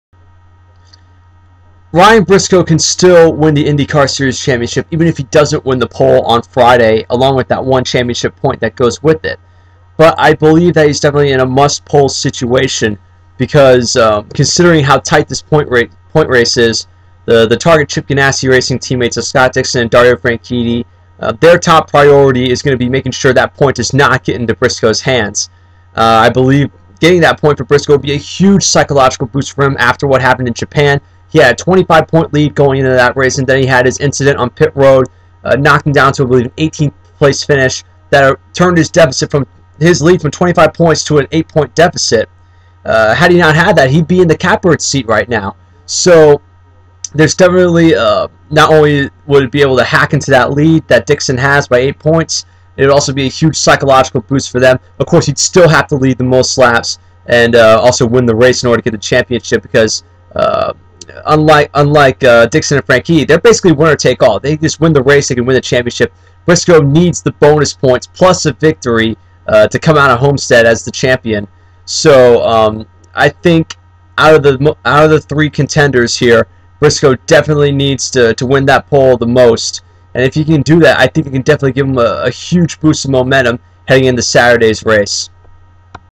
IRR -- Ryan Briscoe and the Homestead pole
Does Team Penske pilot Ryan Briscoe face a "must-pole" situation on Friday at Homestead? The pole position nets a driver one championship point and at eight points back of Scott Dixon with one race left, Briscoe needs every marker he can get! Chris Estrada talks about it.